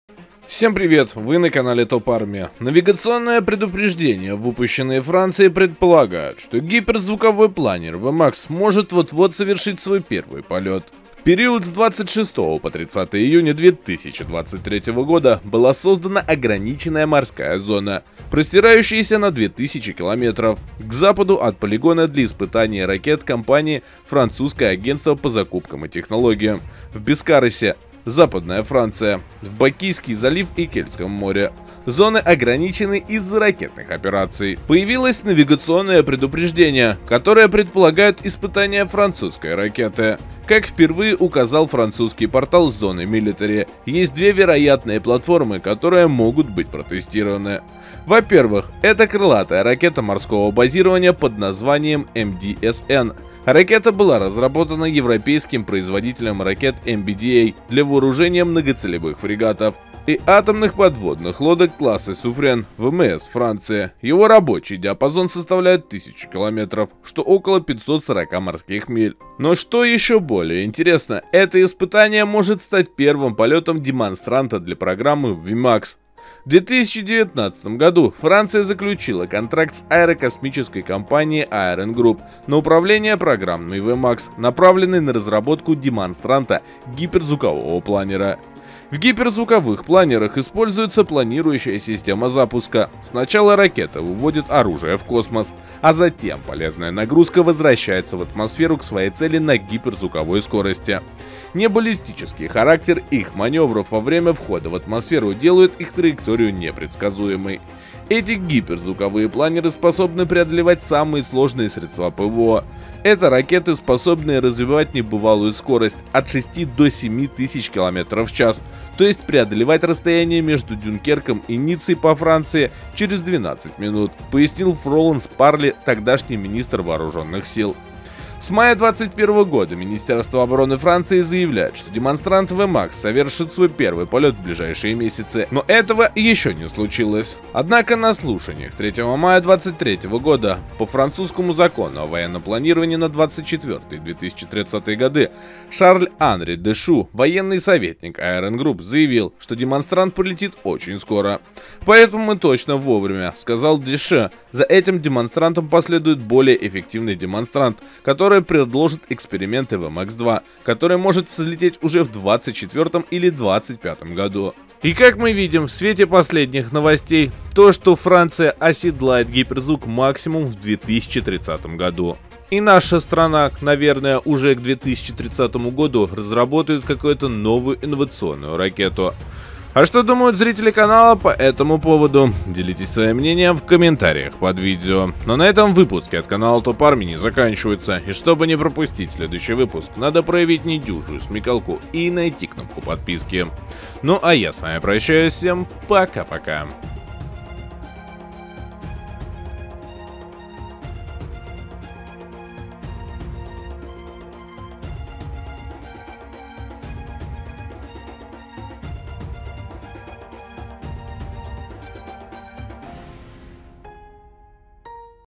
Технологический прорыв! Франция готовится запустить свой революционный гиперзвуковой планер V MAX
В гиперзвуковых планерах используется планирующая система запуска: сначала ракета выводит оружие в космос, а затем полезная нагрузка возвращается в атмосферу к своей цели на гиперзвуковой скорости. Небаллистический характер их маневров во время входа в атмосферу делает их траекторию непредсказуемой.